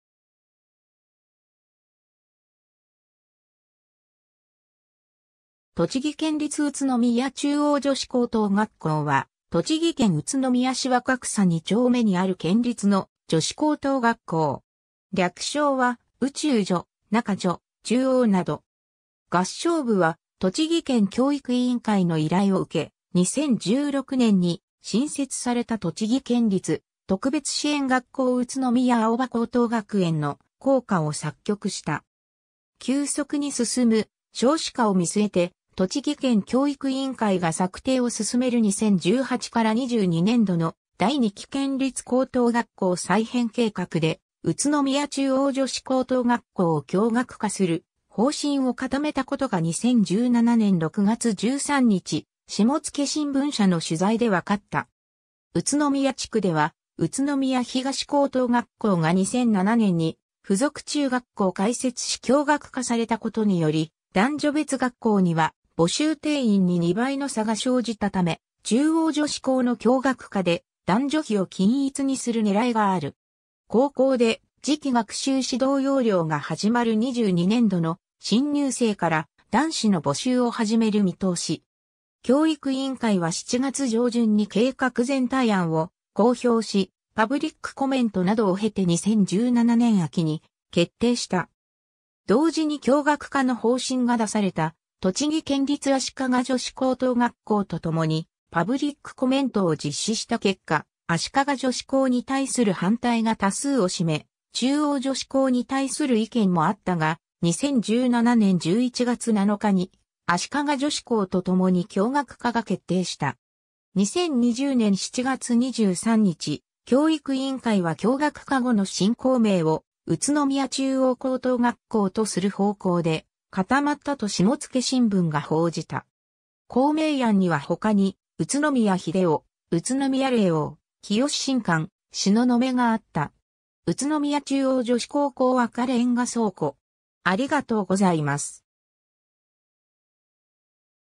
栃木県立宇都宮中央女子高等学校
栃木県立宇都宮中央女子高等学校（とちぎけんりつうつのみやちゅうおうじょしこうとうがっこう）は、栃木県宇都宮市若草二丁目にある県立の女子高等学校。
略称は「宇中女（うちゅうじょ）」、「中女（ちゅうじょ）」、「中央（ちゅうおう）」など。
 合唱部は栃木県教育委員会の依頼を受け、2016年（平成28年）に新設された栃木県立特別支援学校宇都宮青葉高等学園の校歌を作曲した。
 急速に進む少子化を見据えて栃木県教育委員会が策定を進める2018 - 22年度の第2期県立高等学校再編計画で、宇都宮中央女子高等学校を共学化する方針を固めたことが2017年6月13日、下野新聞社の取材で分かった。
宇都宮地区では、宇都宮東高等学校が2007年に附属中学校開設し共学化されたことにより、男女別学校には募集定員に2倍の差が生じたため、中央女子高の共学化で男女比を均一にする狙いがある。
高校で次期学習指導要領が始まる22年度の新入生から男子の募集を始める見通し。
教育委員会は7月上旬に計画全体案を公表し、パブリックコメントなどを経て2017年秋に決定した。
 同時に共学化の方針が出された栃木県立足利女子高等学校とともにパブリックコメントを実施した結果、足利女子高に対する反対が多数を占め、中央女子高に対する意見もあったが、2017年11月7日に足利女子高とともに共学化が決定した。
2020年（令和2年）7月23日、教育委員会は共学化後の新校名を「宇都宮中央高等学校」とする方向で固まったと下野新聞が報じた。
校名案にはほかに宇都宮秀央（しゅうおう）、宇都宮令央（れいおう）、潔進館（けっしんかん）、東雲（しののめ）があった。
 宇都宮中央女子高校赤レンガ倉庫（旧第六十六歩兵連隊倉庫）